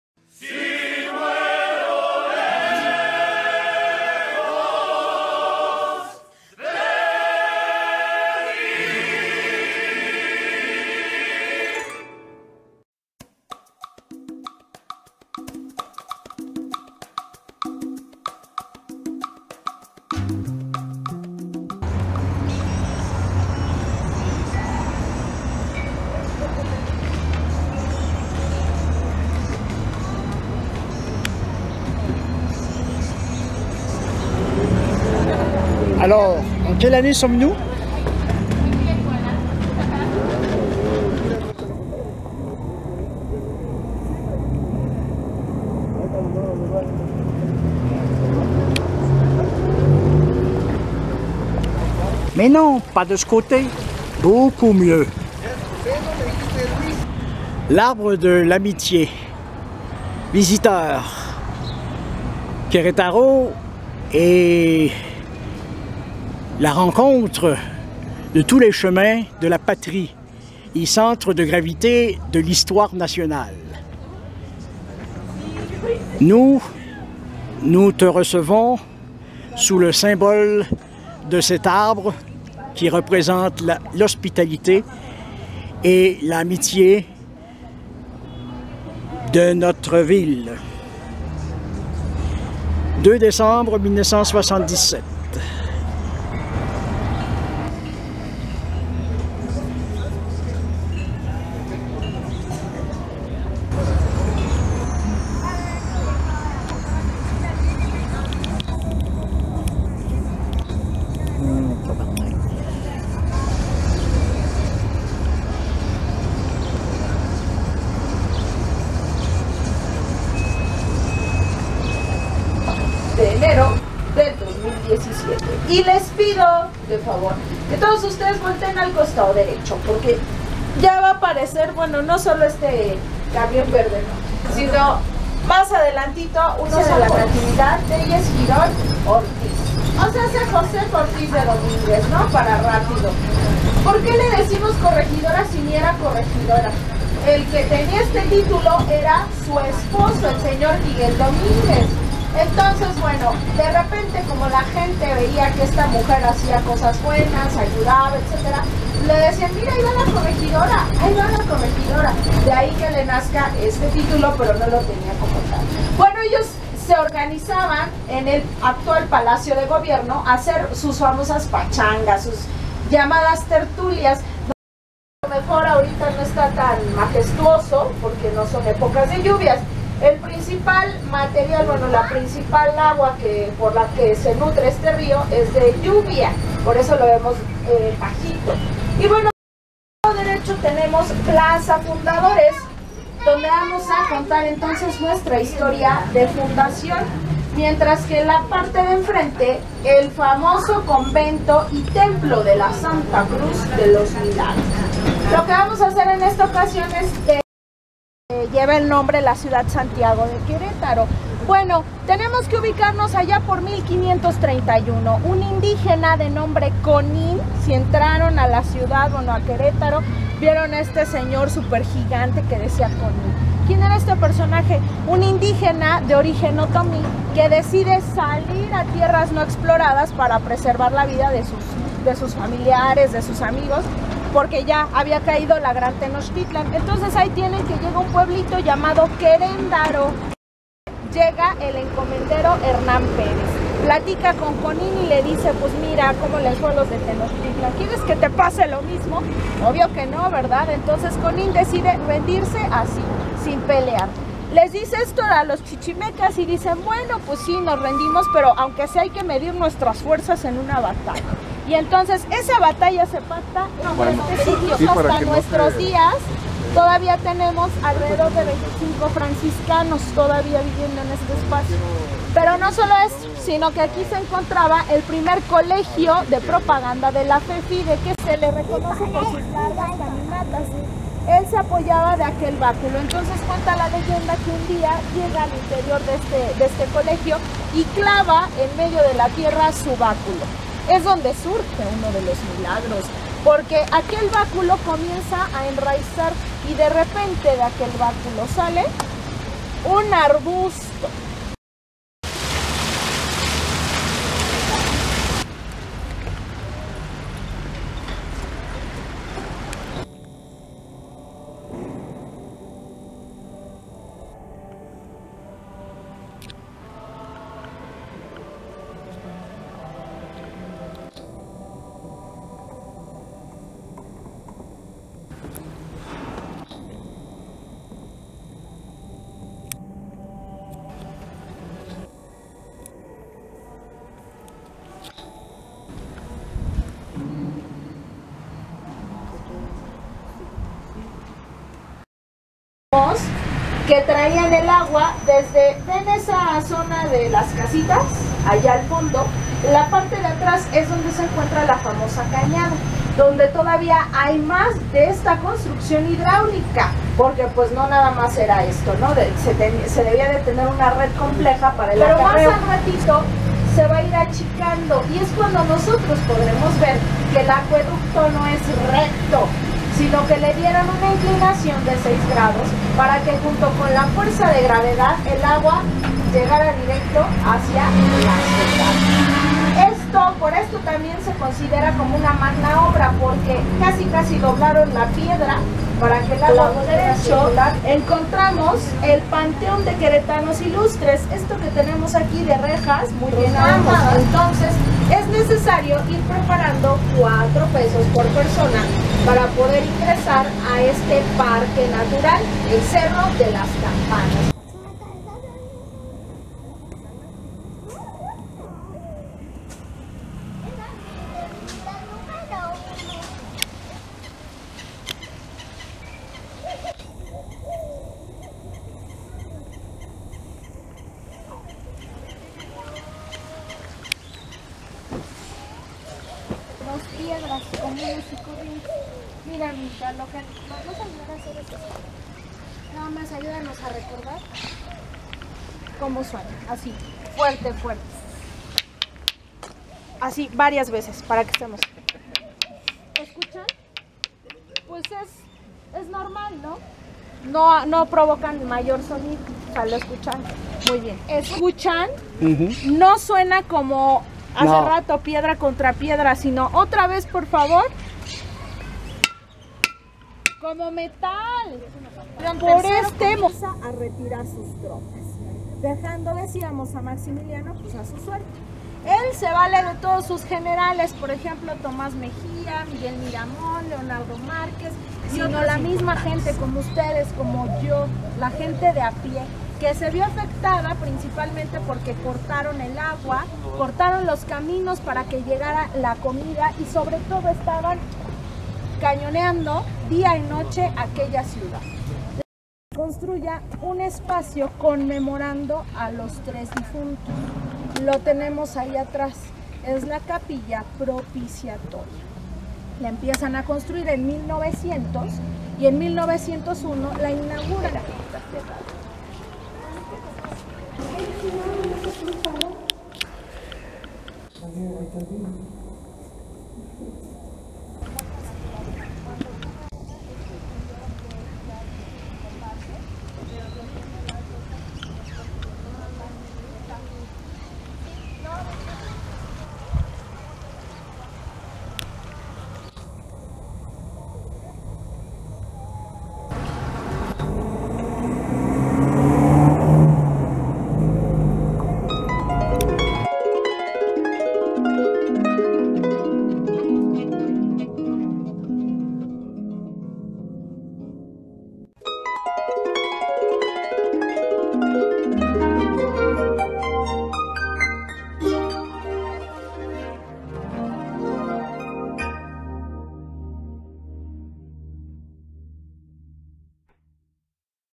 Un tour de Queretaro très instructif
Queretaro est une ville qui recèle des centaines de pages de l'histoire du Mexique. On en découvre a pied mais aussi lors d'un tour dans un de ces vieux tramways neufs. L'aqueduc est un élément fort intéressant de l'histoire toujours bien conservé.